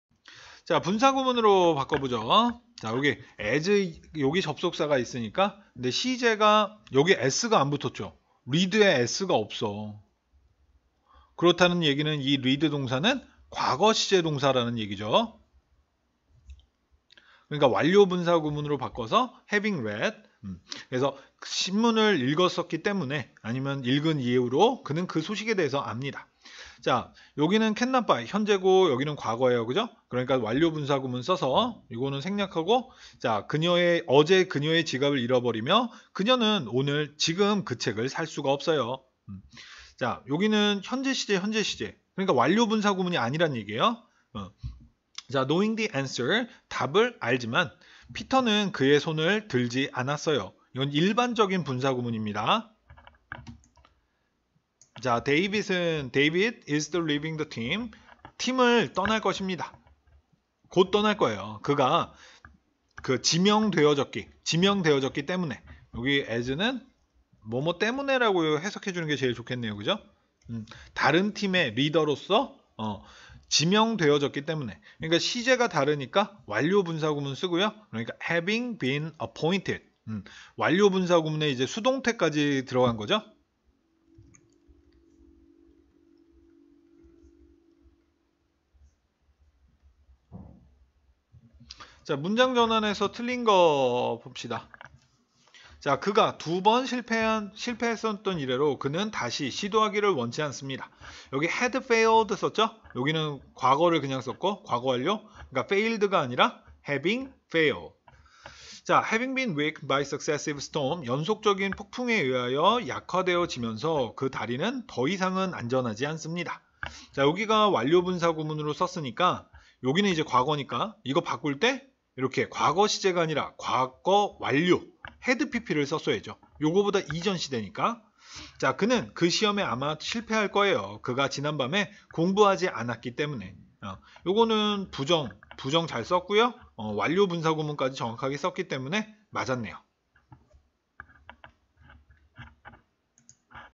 1-4탄_05.04.mp4
중등 내신 영문법에 특화된 1탄 포인트 문법의 연습문제 1-4탄